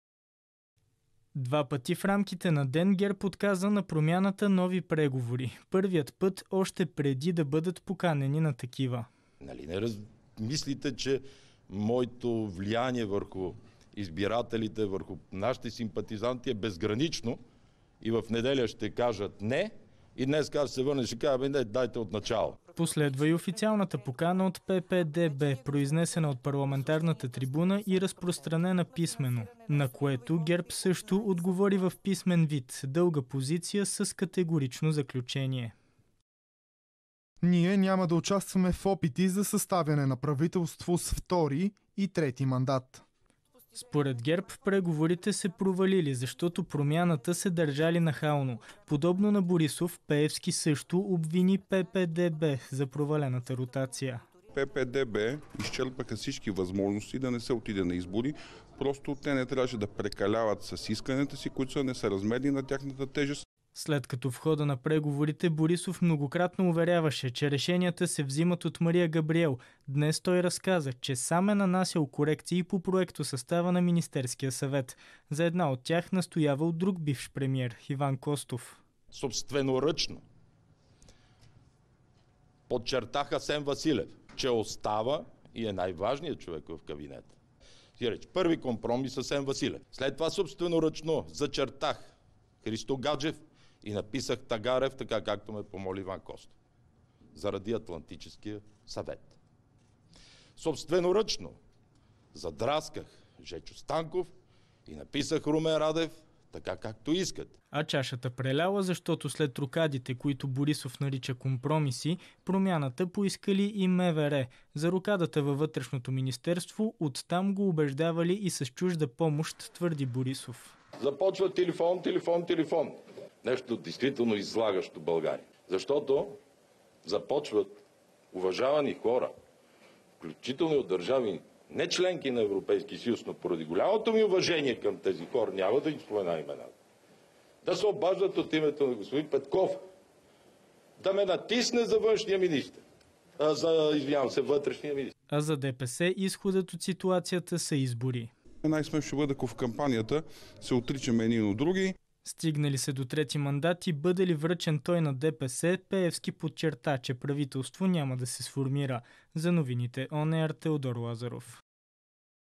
Борисов затвори вратата: Няма да има преговори при втория мандат за кабинет
От ПП-ДБ излязоха с предложение към ГЕРБ за Национален мандат за правителство. Още преди Атанас Атанасов да отправи предложението за общ втори мандат към ГЕРБ, лидерът на партията Бойко Борисов отклони все още неполучената тогава покана.

"Не ви ли се струва нахално след всичките тези преговори да кажат "Връщаме се, всички министри са наши, айде сега дайте ни регулаторите и службите и всичко останало"?! Нали не мислите, че моето влияние е безгранично и в неделя ще кажем "не", а днес ще кажа дайте отначало", заяви Борисов.

Подобно на Борисов, Делян Пеевски също обвини "Продължаваме промяната" за провала на първия мандат.